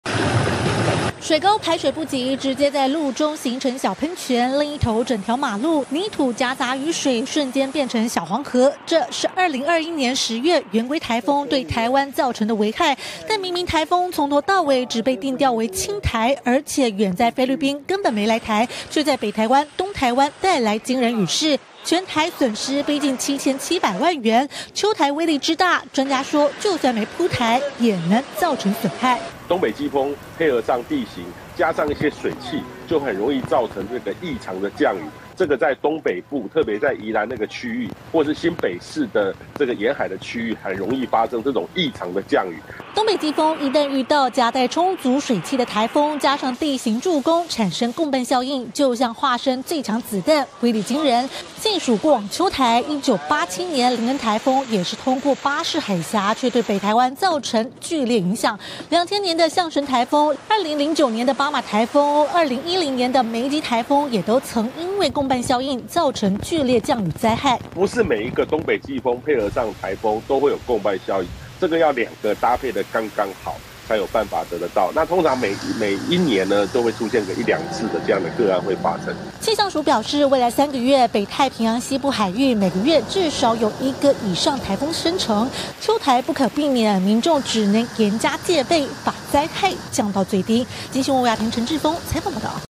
秋颱＋東北季風若形成「共伴」 異常降雨致災｜鏡新聞
就算進入秋季，氣象署預估未來三個月，北太平洋西部海域，每個月至少還是會有一個以上颱風生成！而「秋颱」的威力，會對台造成多大影響？氣象專家說，關鍵就得看，它和東北季風的相互配合，一旦兩者形成「共伴效應」，威力將會很驚人，就算颱風沒有直撲台灣，也可能出現劇烈降雨，像是過往的琳恩、圓規，這幾個秋颱，都對台灣造成嚴重損害。